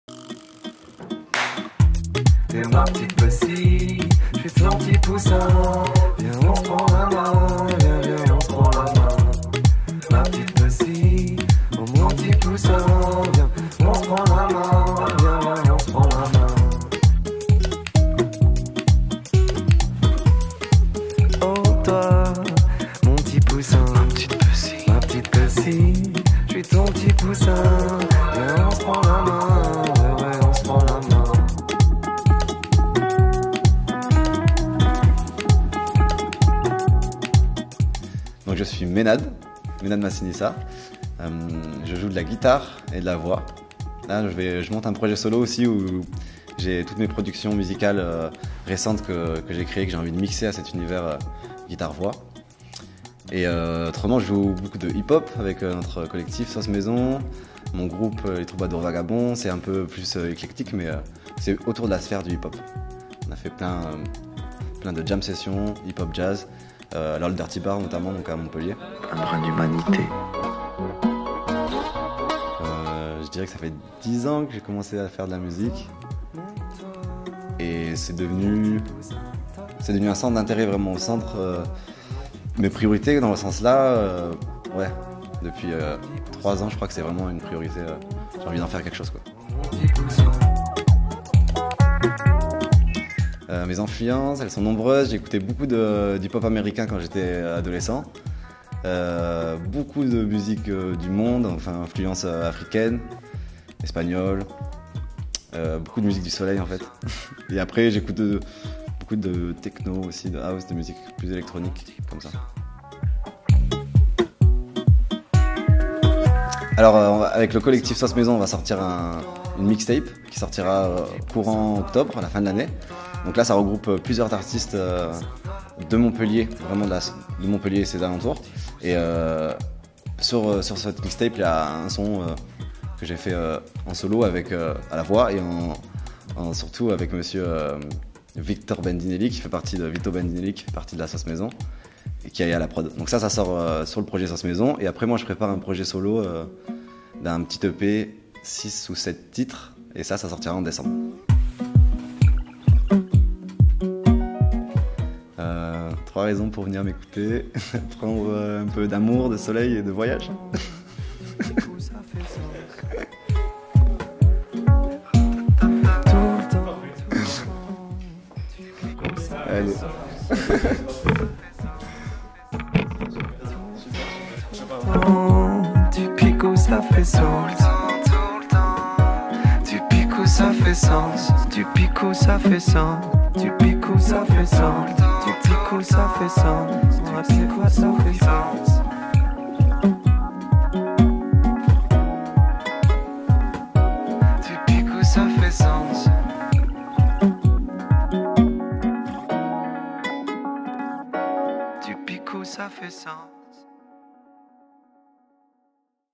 Massinissa: OFF
Entre Interview et Making Of, retrouvez Mennad Massinissa avant la sortie de sa session live sur K7. Il nous y présente ses projets, son collectif, ses influences et sa passion.

À très vite sur K7.